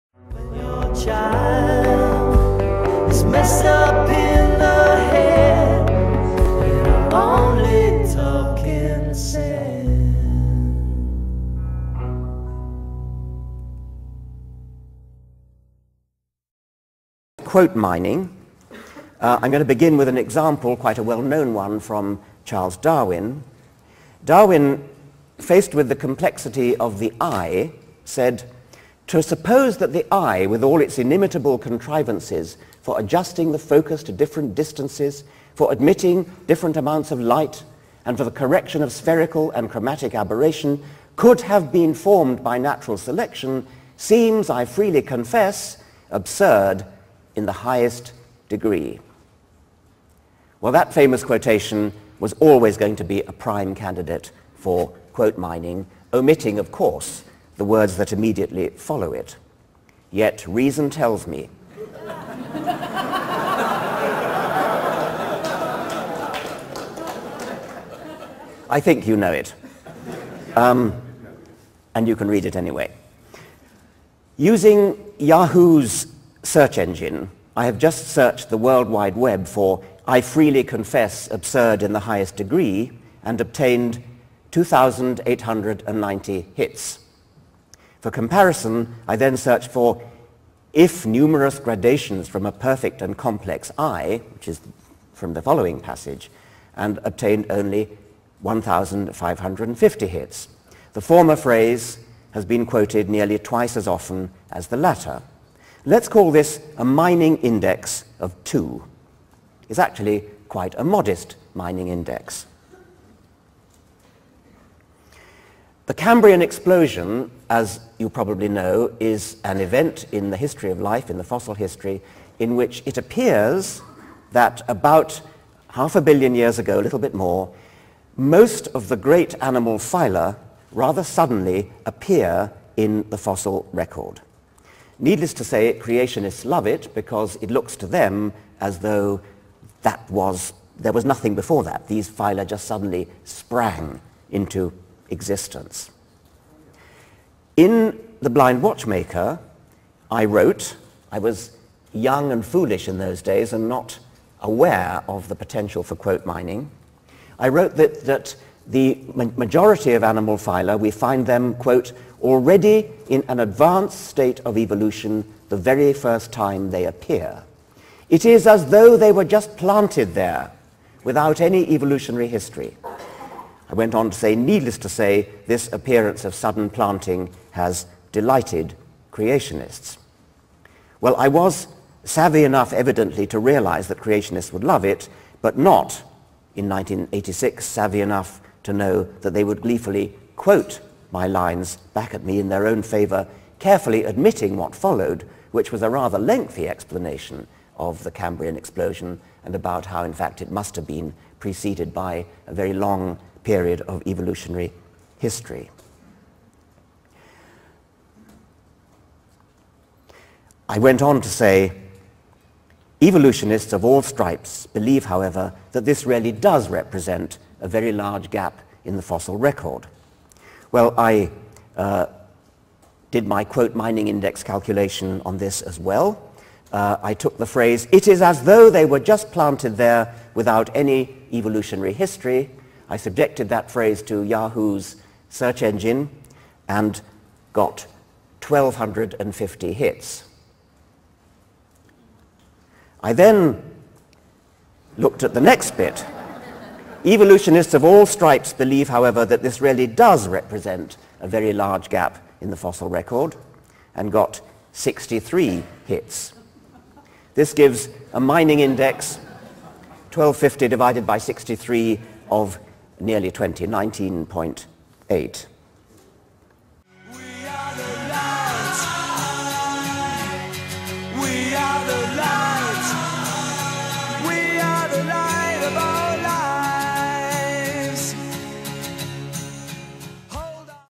Richard Dawkins on The Art of Quote Mining - Part 1
Richard Dawkins provides a couple of examples of popular quote mines in the Evolution/Creation/ID debate. One is a popular one from Charles Darwin on the eye. The other is from professor Dawkins himself on the Cambrian Explosion.

Quote mining is a deeply dishonest practice leading, as it does, to the repetition of the lie by the unwitting lay-person.